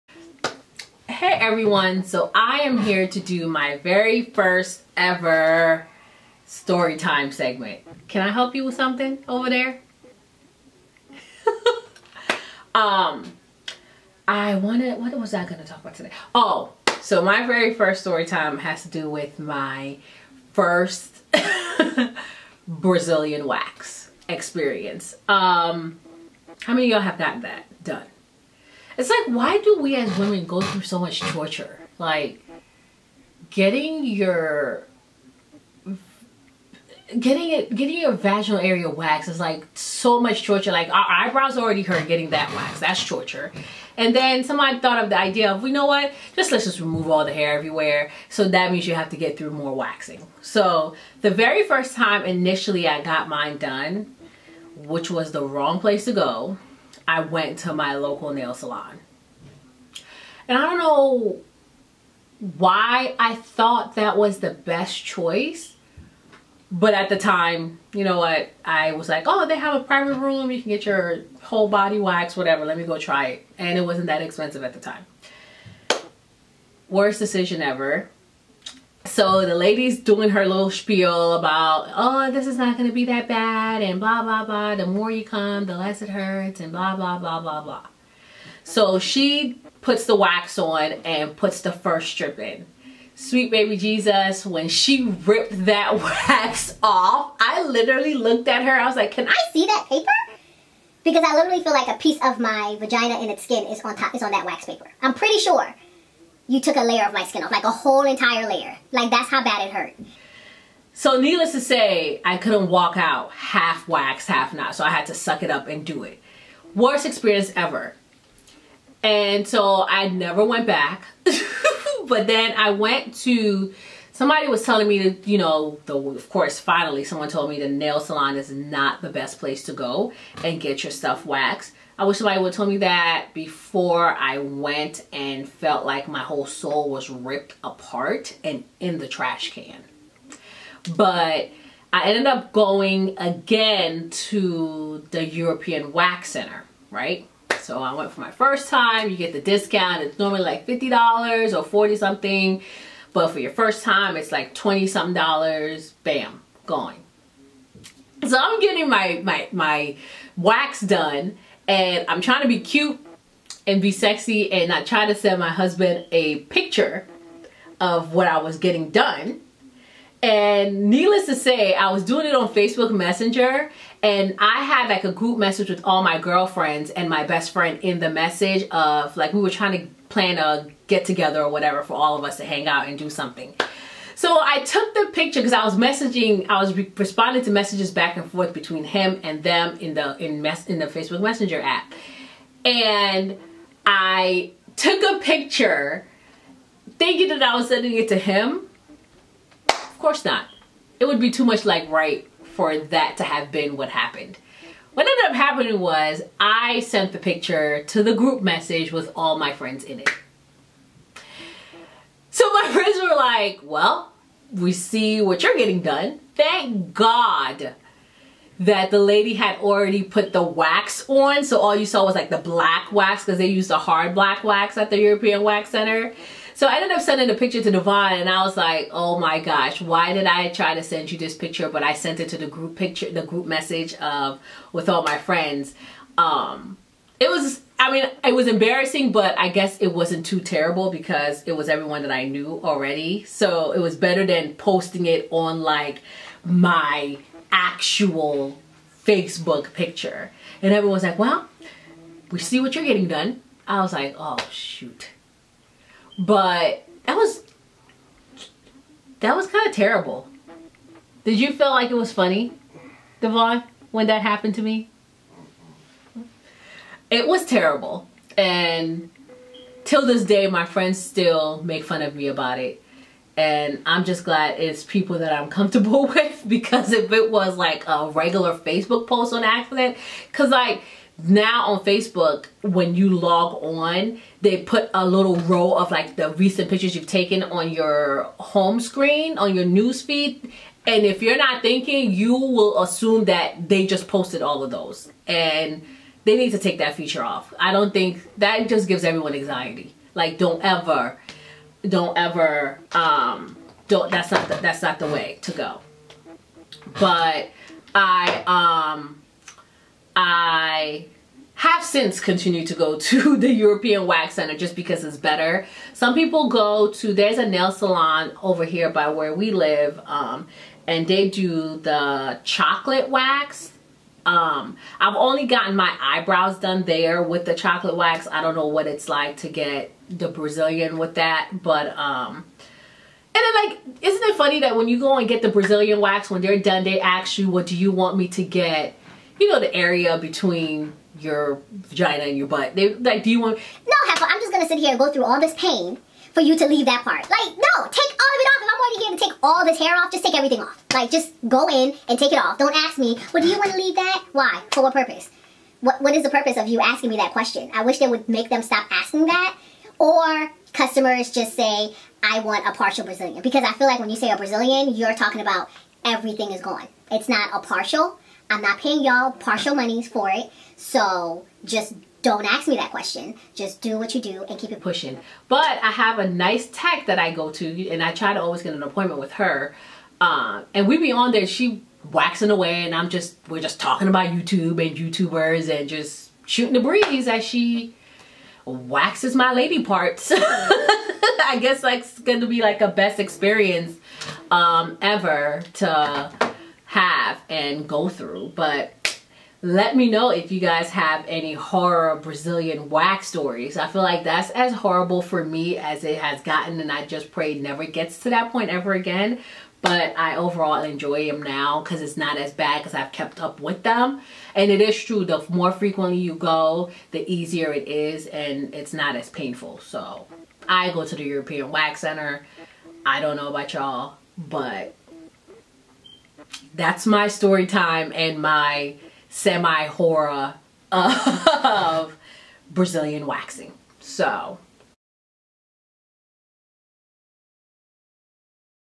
Storytime:First Brazillian wax|SLVIXXEN
*READ ME***
Head about my first Brazilian wax experience
***Find me**